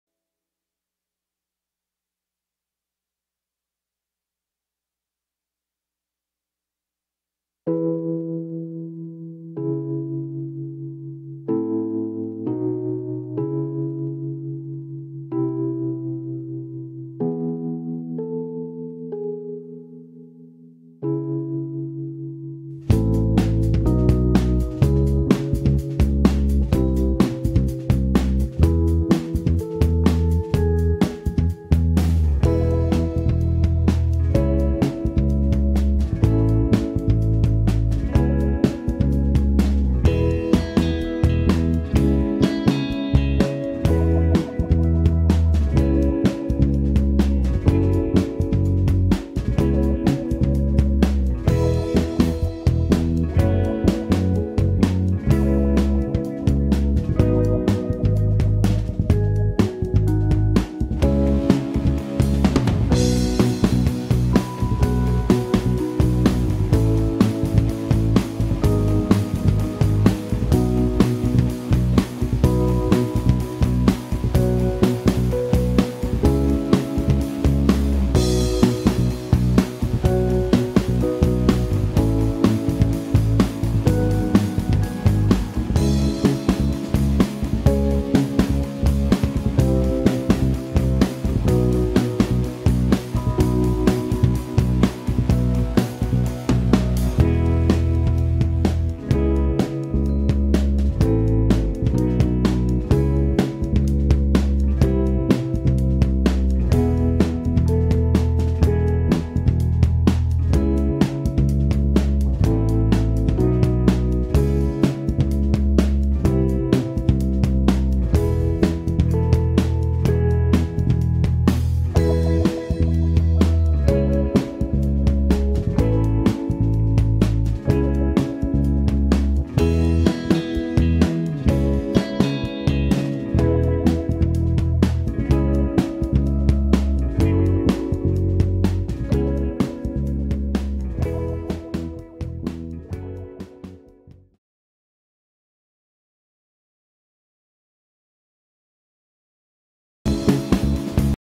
Ardnaxela back in the studio*2009
Behind the scenes de el proceso de la nueva grabacion discografica de Ardnaxela en el Studio 666 en Mexico City.
Alex Gomez en drums, Chuck en bass, Tweety on synth, Matute on keyboards, Rick Deliz on guitars...coming soon...